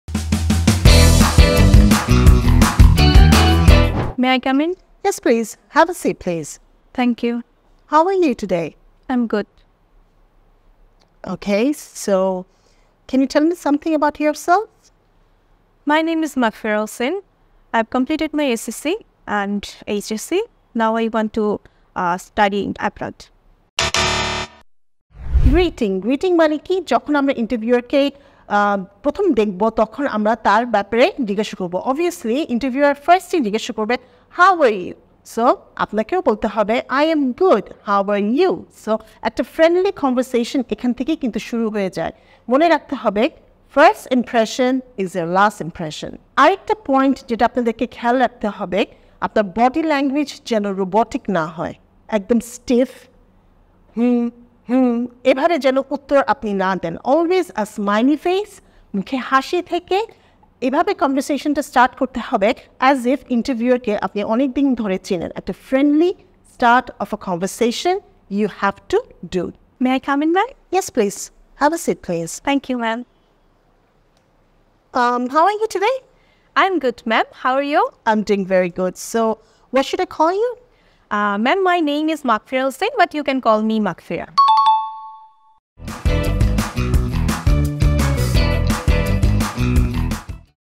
IELTS Speaking Tip: Avoid Casual Greetings for a Better First Impression!
Quick IELTS Speaking Tip! Starting with 'Hey' or 'What's up?' might feel natural, but it's better to keep it formal. A simple 'Good morning' or 'Good afternoon' makes a better impression!